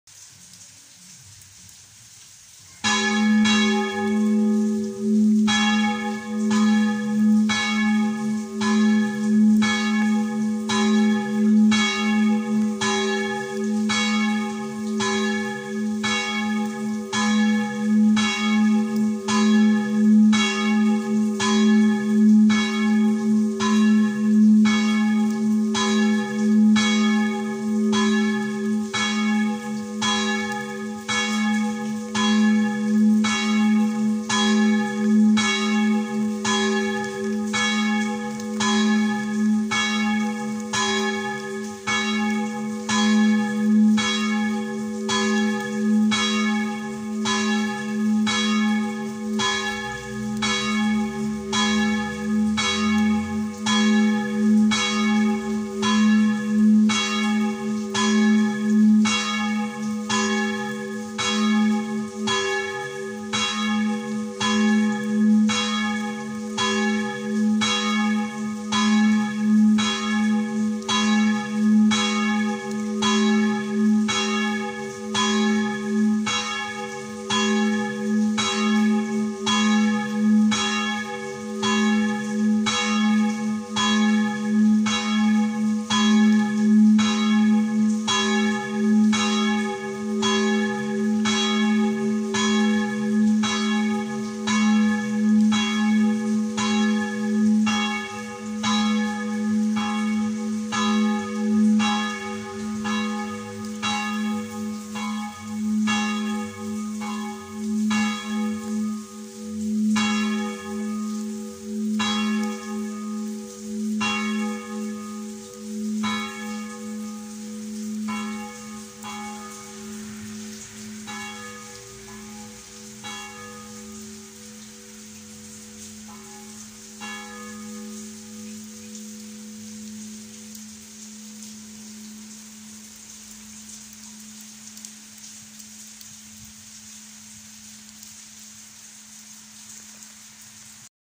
campana piccola di Porpetto
Tre campane in mib3 fuse da f.broili nel 1921, grande rifusa nel 1944 e piccola nel 1958